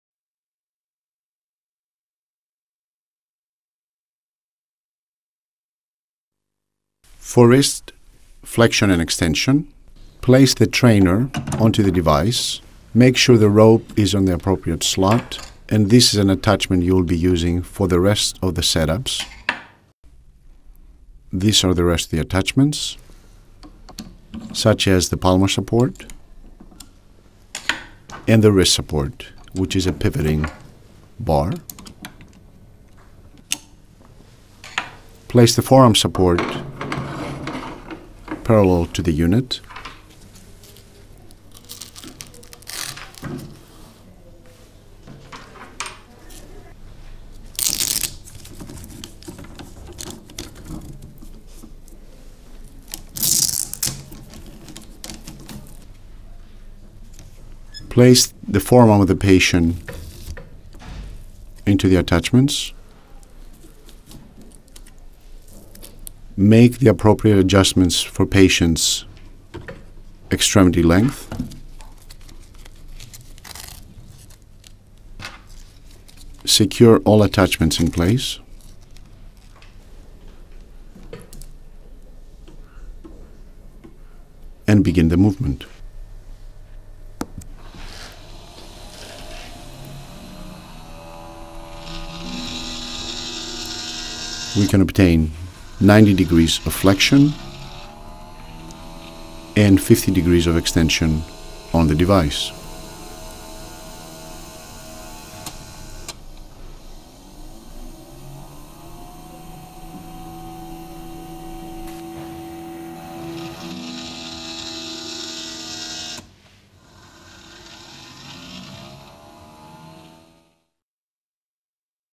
Kinetec Maestra™ hand & wrist CPM -wrist flexion/extension-
Tutorial on how to set up the wrist flexion/extension movement on the Kinetec Maestra™ CPM